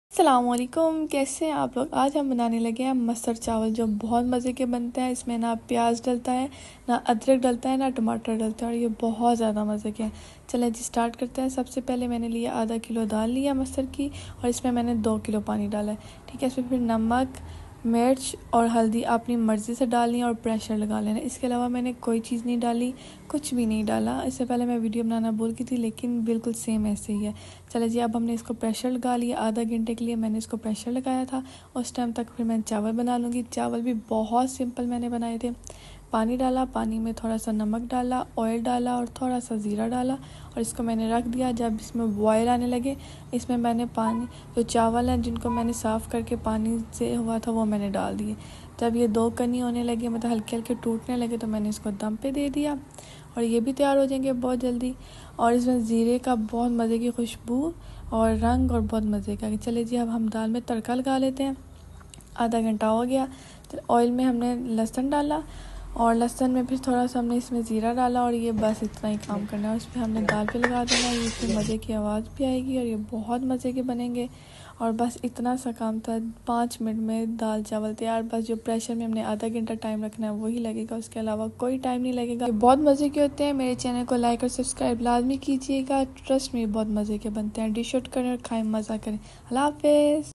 masar chawal quick recipe || گھر میں مزے دار مسر چاول تیار کریں || na پیاز na tamatar na ادراک
In this vedio you will see quick recipe
Masar chawal
Step by step
Na payaz
Na adrak
Na tamatar
Or daal tayar
Danya or gharam masala dalna hy
Trending recipe